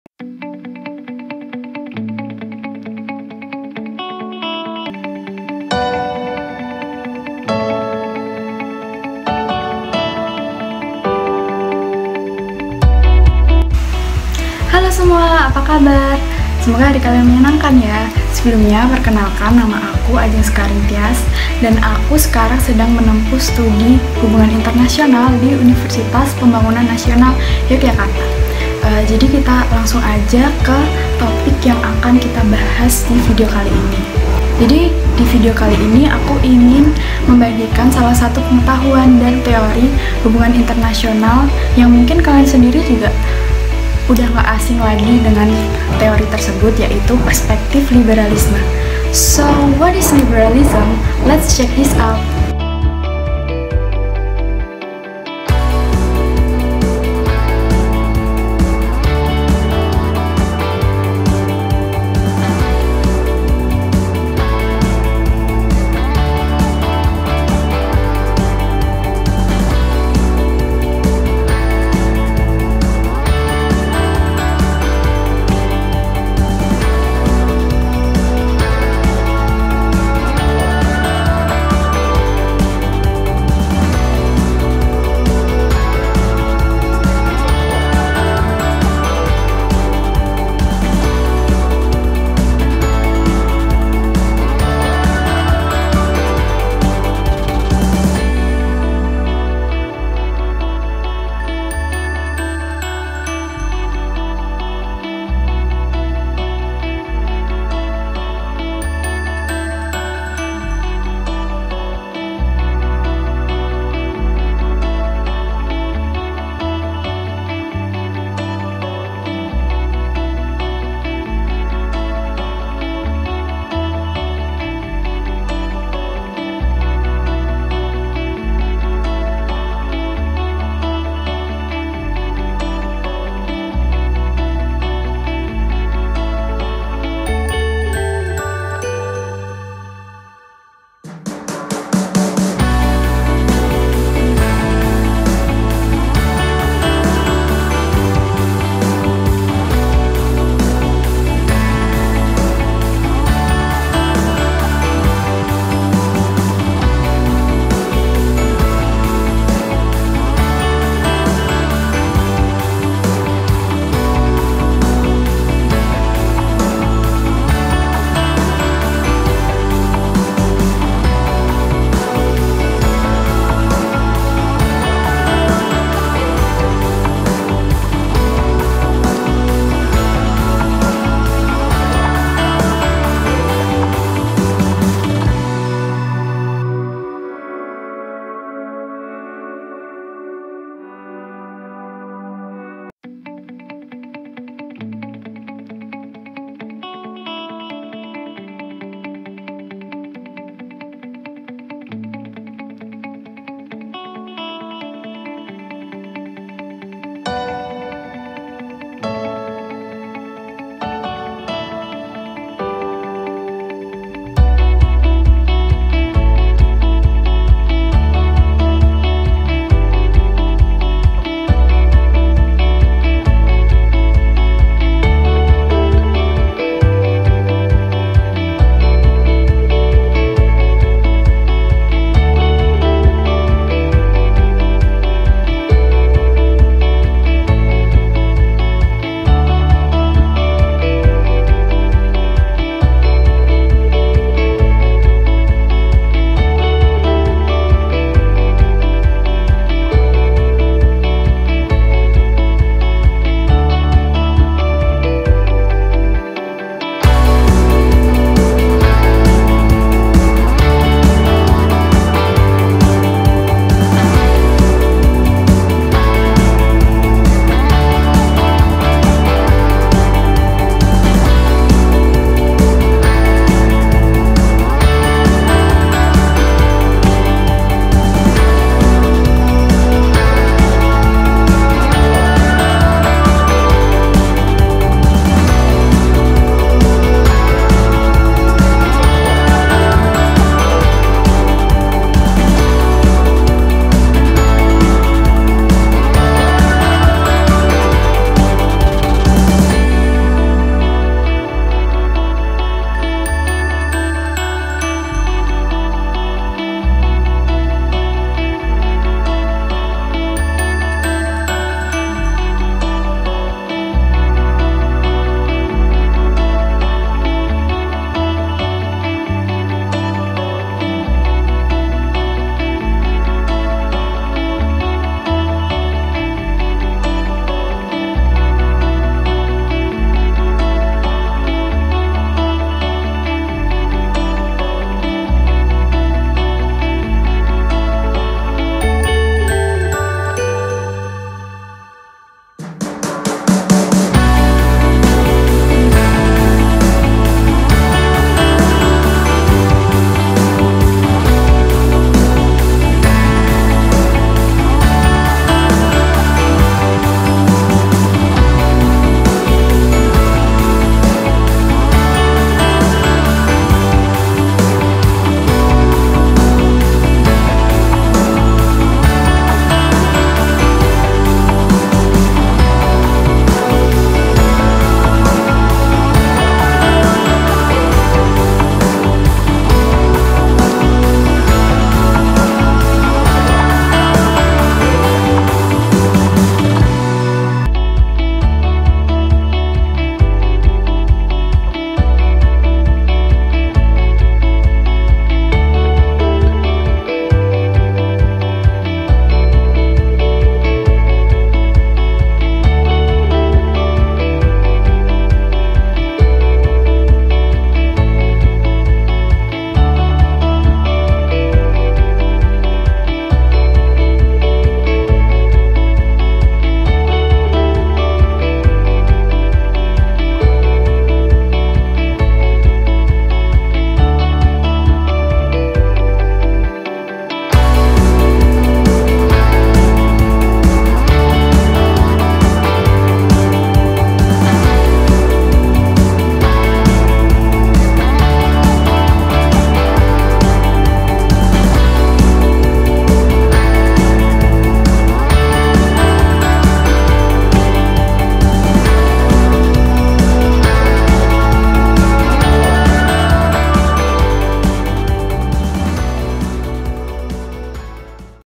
LIBERALISM IN INTERNATIONAL RELATIONS
The video about Liberalism, this video is an assignment for the final semester exam of study Theory of International Relations.
Lecturer of this study, Dr. Machya Astuti Dewi, M. Si.
International Relations of UPN "Veteran" Yogyakarta.
2020.